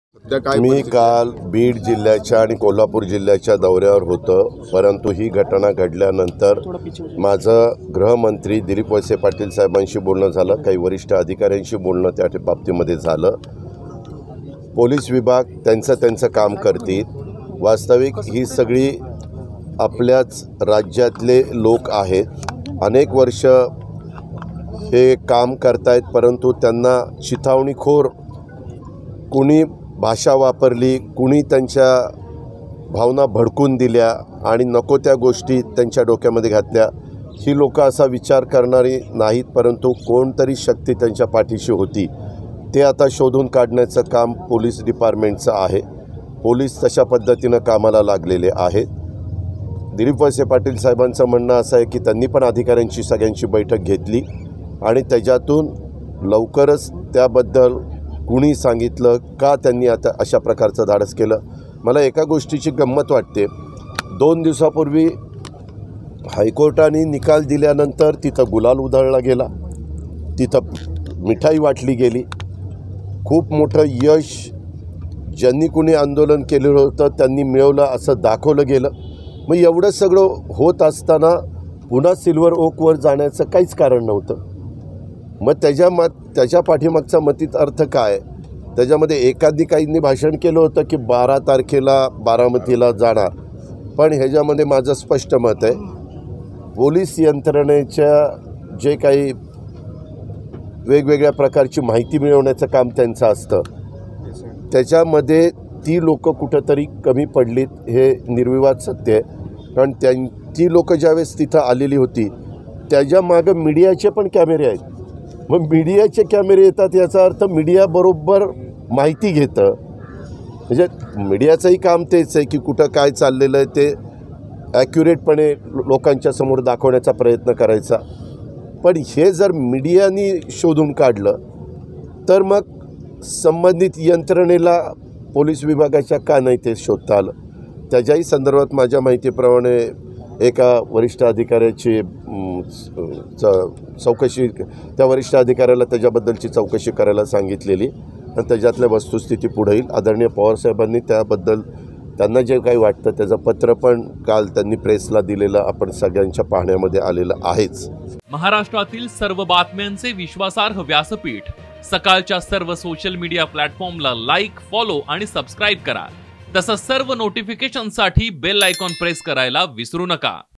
Silver Oak Attack: पोलिसांना हे आधी का कळलं नाही? अजित पवार संतापले | Ajit Pawar | Sakal
राष्ट्रवादी काँग्रेसचे ज्येष्ठ नेते शरद पवार यांच्या मुंबईतील घरावर शुक्रवारी संतप्त एसटी कर्मचाऱ्यांनी उग्र आंदोलन केले. राज्याचे उपमुख्यमंत्री अजित पवार यांनी देखील या घटनेविषयी तीव्र नाराजी व्यक्त केली. पोलिसांना हे आधी का कळले नाही, पोलीस यंत्रणेचं हे अपयश आहे असं म्हणत त्यांनी आपली खंत व्यक्त केली.